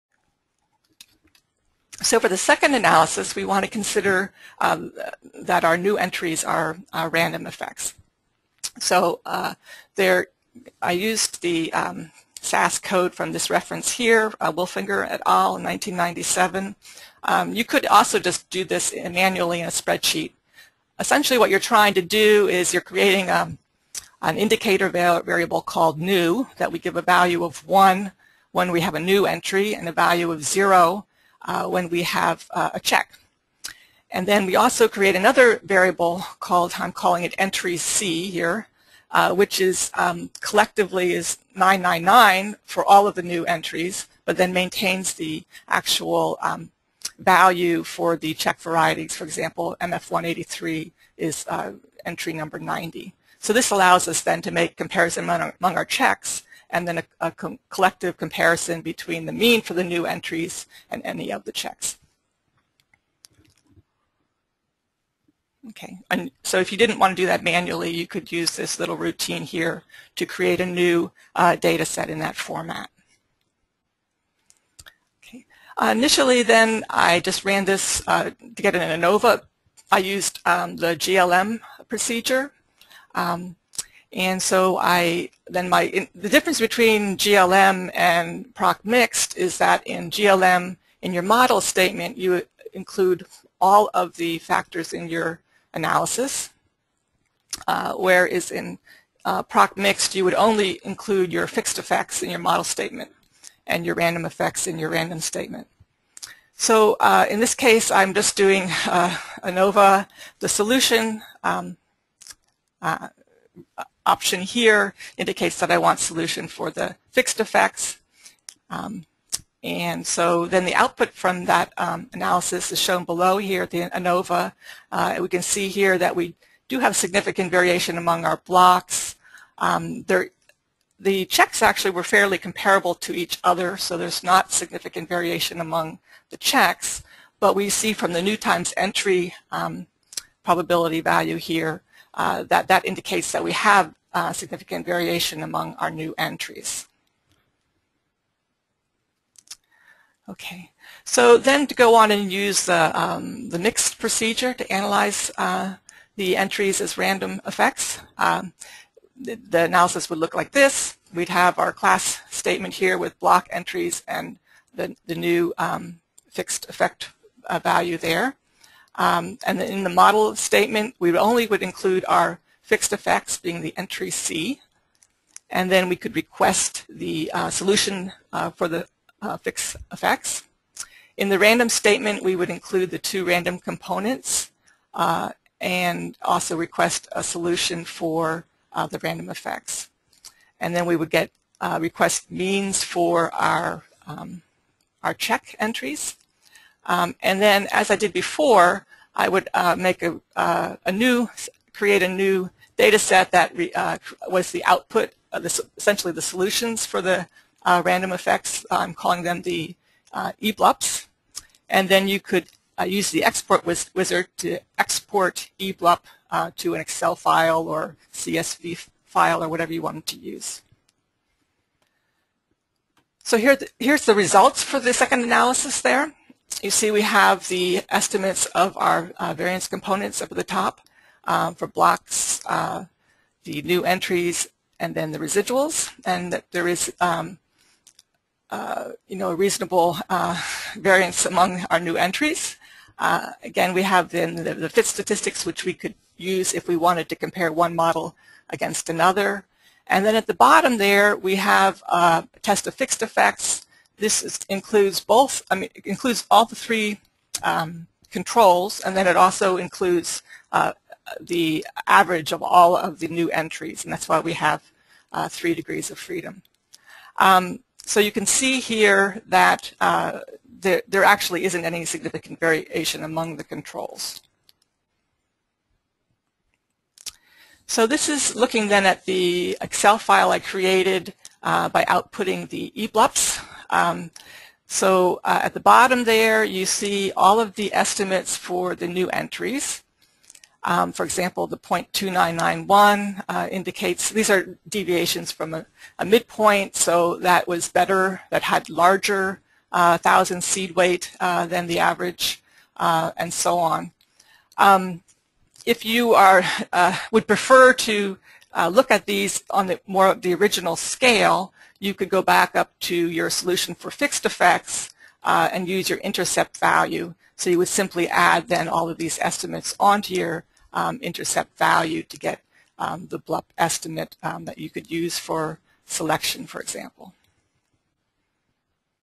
Introduction to the Augmented Experimental Design Part 5 of 8
Part 5 of 8. Analysis treating genotypes as random. Learn how to design experiments and analyze data using an augmented design. This introductory webinar was presented by Dr. Jennifer Kling, Oregon State University and hosted by Dr. Heather Merk, Ohio State University. Dr. Kling's presentation is available in pdf and video formats. In addition, the data set and SAS code used in this webinar are available